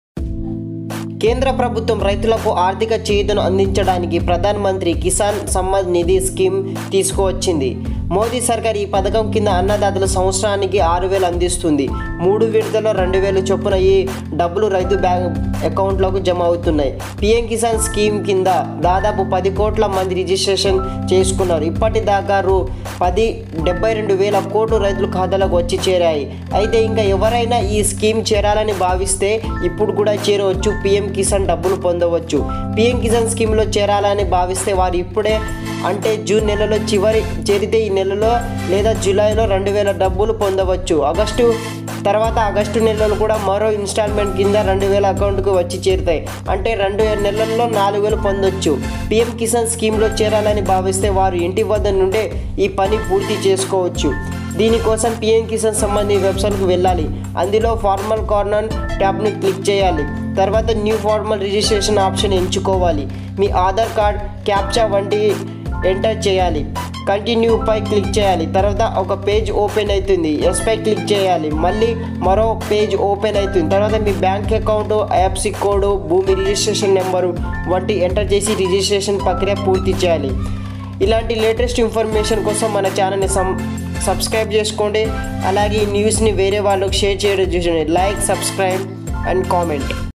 PM- KISAN Money New Registration 2020 | ఇప్పటివరకు పీఎం కిసాన్ డబ్బులు రాని వాళ్ళు అప్లై చేసుకోవచ్చు
Apply New Registration

Pm Kisan Money Status Check